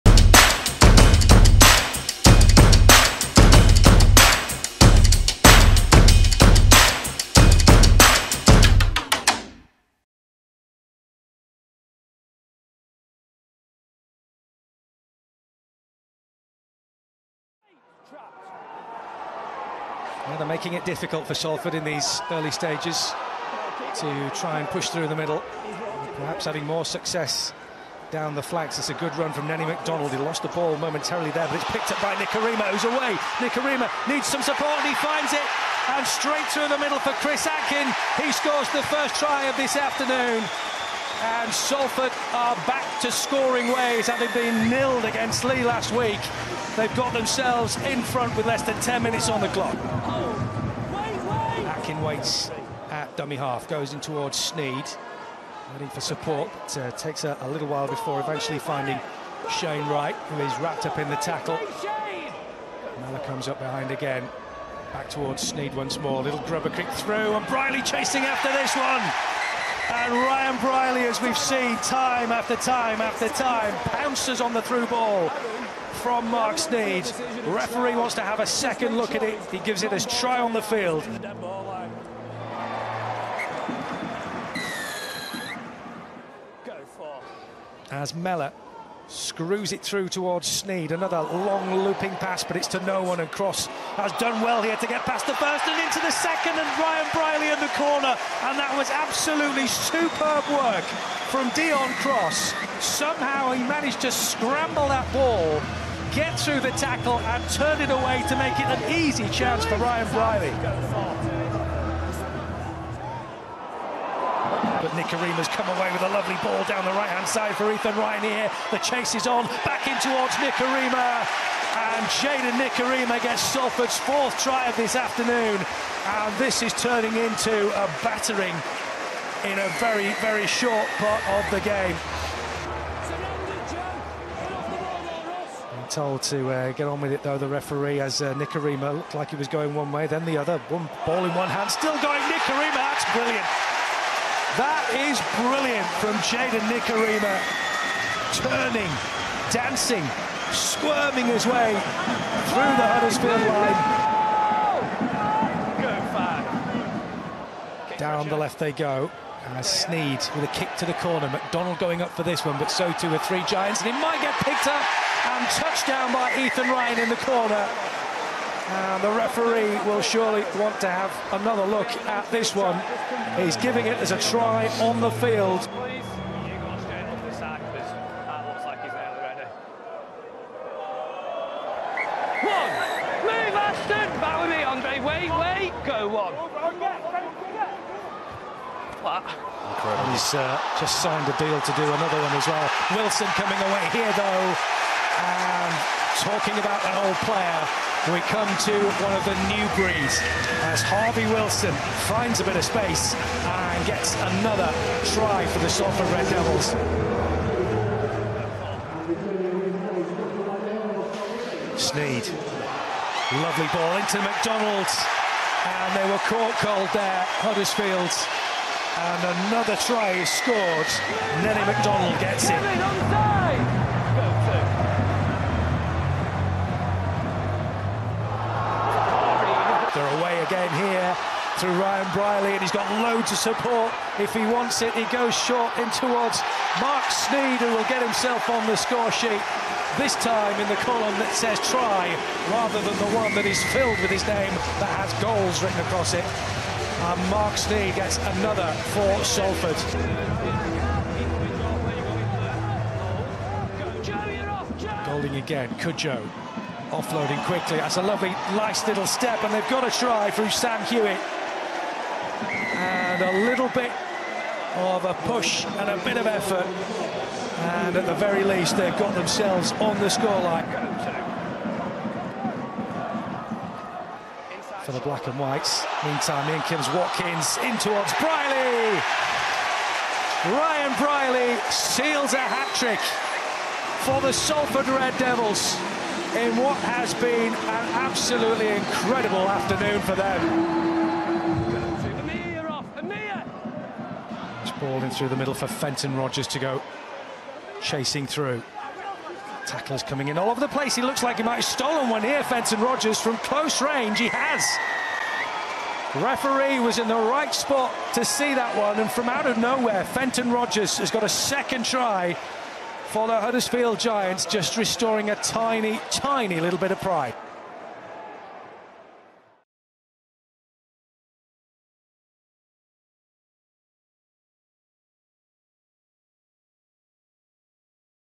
Highlights | Salford Red Devils v Huddersfield Giants | 2024 Betfred Super League, Round 23
Watch all the highlights as Salford Red Devils entertained Huddersfield Giants in Round 23 of the Betfred Super League!